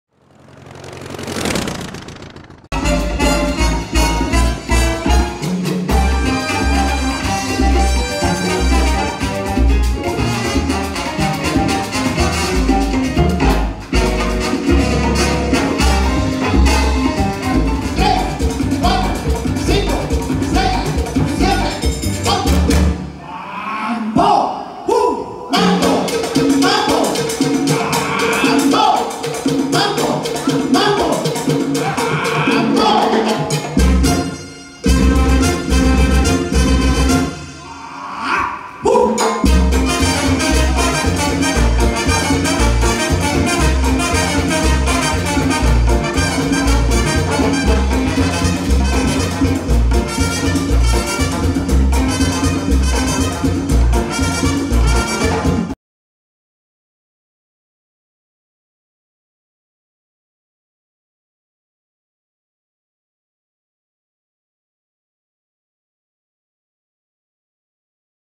Dance show at Buena Vista Social Club in Havana, Cuba
Buena Vista Social Club in Havana Cuba.  World famous dance show. Cuban dance styles are Danzon, Mambo, cha-cha-cha, bolero, salsa, and ballet.

This is a historic venue that is internationally known as having one of the best dance shows in the world.  Check out the nightly dancing performance in La Habana, Cuba